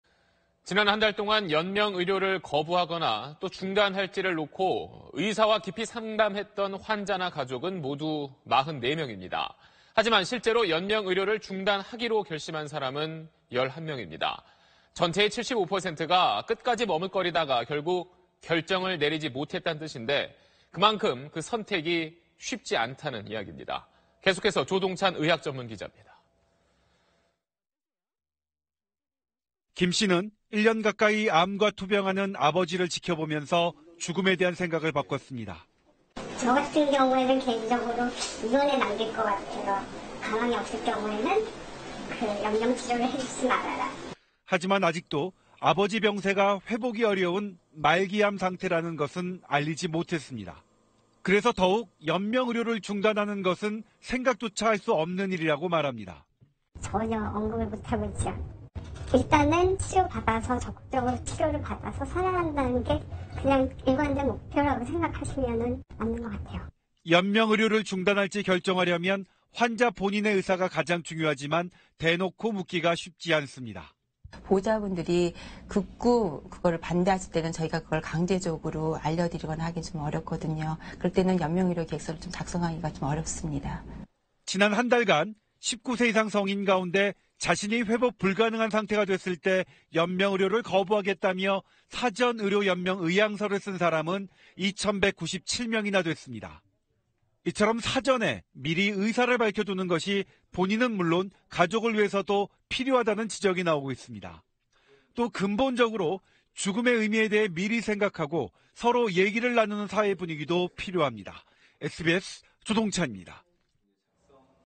존엄사 기로에서 75%는 '머뭇'…허용 확대 추진 / SBS
지난 한 달 동안 연명 의료를 거부하거나 중단할지를 놓고 의사와 깊이 상담했던 환자나 가족은 모두 44명입니다. 하지만 실제 연명 의료를 중단하기로 결심한 사람은 11명입니다. 전체의 75%는 끝까지 머뭇거리다 결국 결정을 내리지 못했다는 뜻인데 그만큼 그 선택이 쉽지 않다는 이야기입니다.

계속해서 조동찬 의학전문 기자입니다.